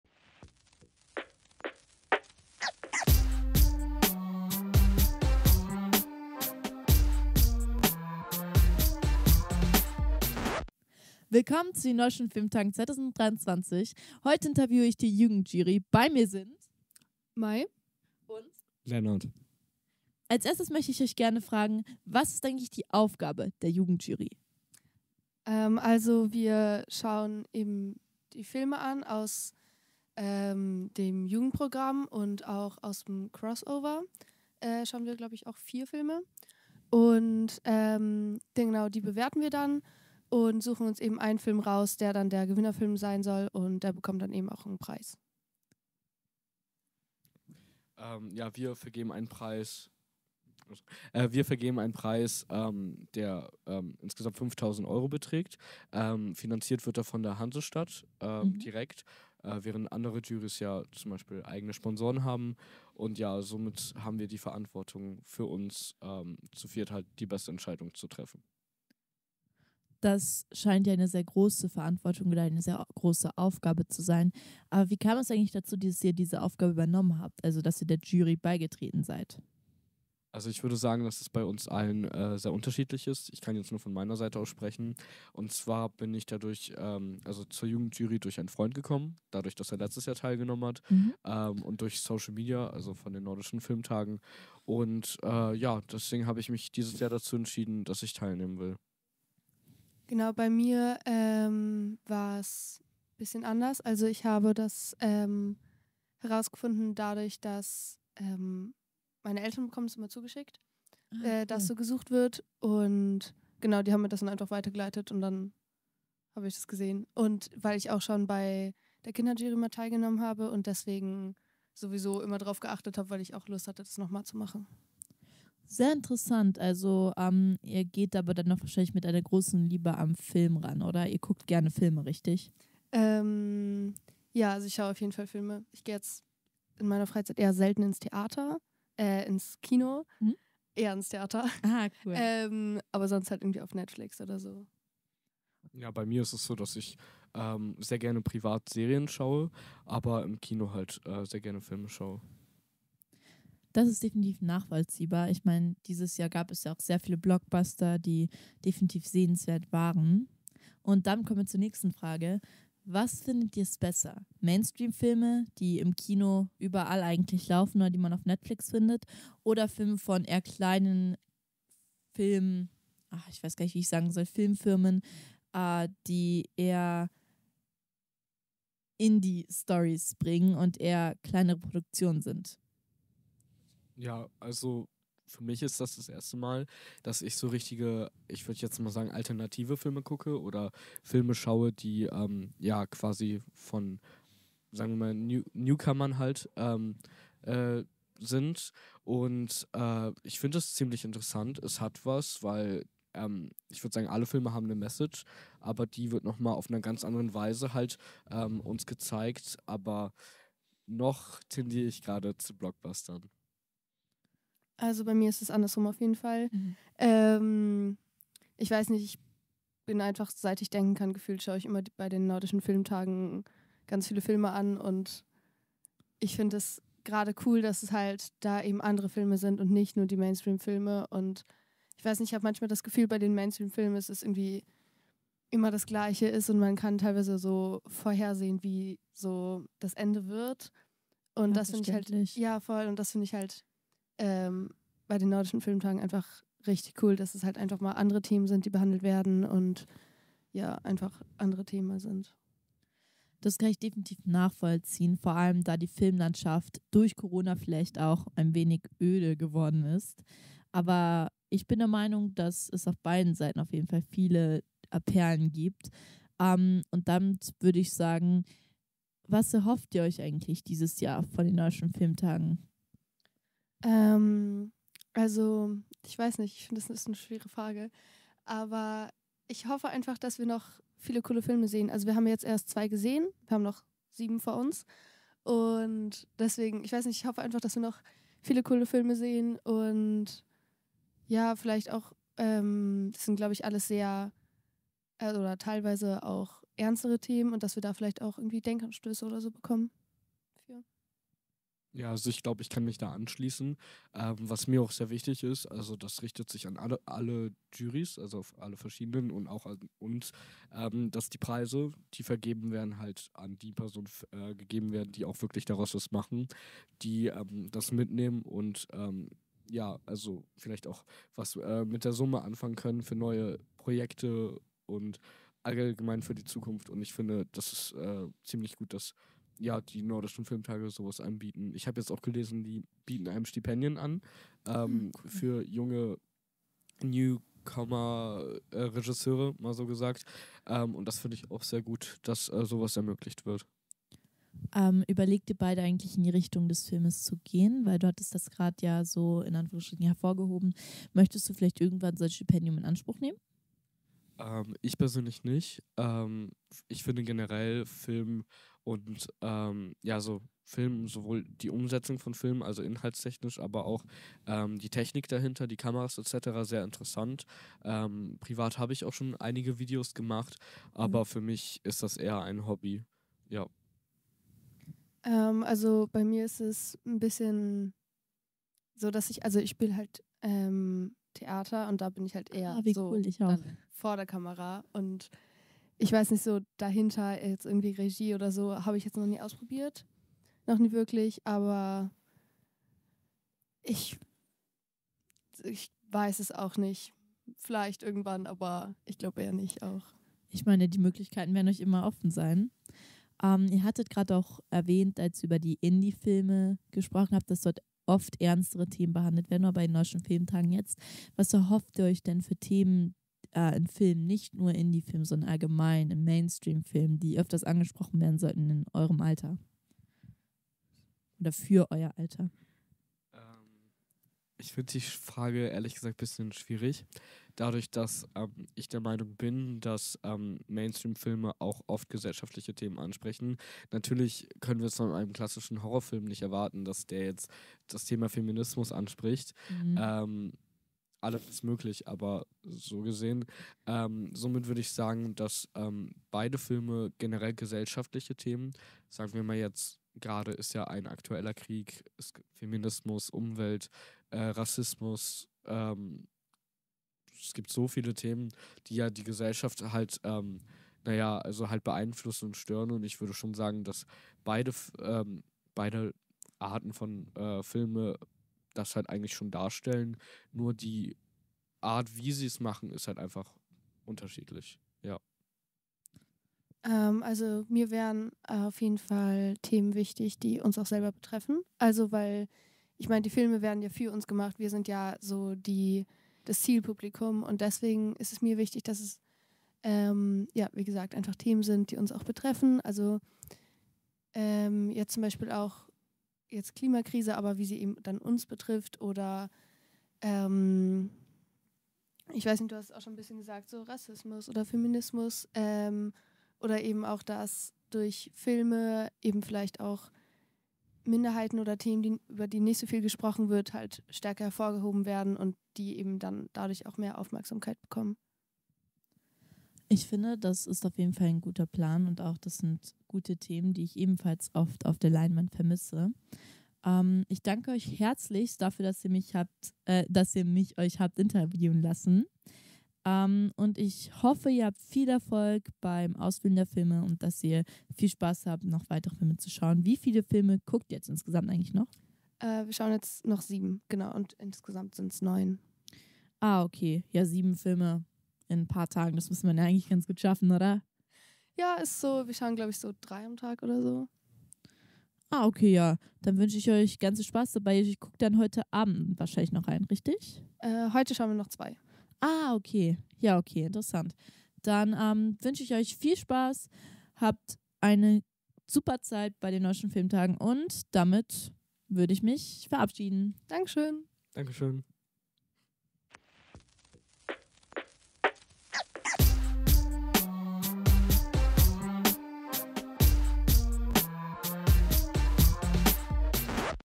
Unser Interview mit der Jugend-Jury 2023 I Junge Festival-Blogger:innen
Was macht man als Jugend-Jury-Mitglied eines Filmfestivals? Die jungen Festival Blogger:inner interviewen Jury Mitglieder Maybritt und Lennart. Erfahre mehr über die spannenden Aufgaben der Jury!